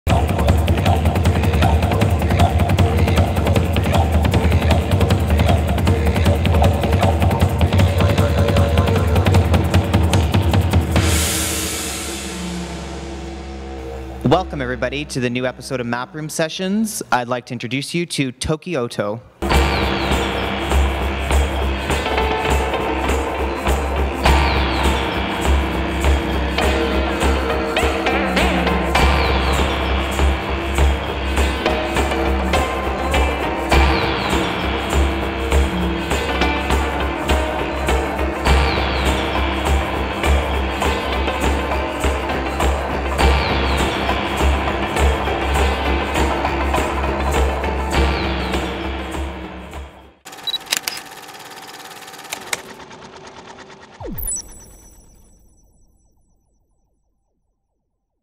Map Room Sessions - Toki Oto (Preview)
Our third and most unique yet Map Room Session is coming out this Wednesday, May 9th: didgeridoo and wave drum duo TOKI OTO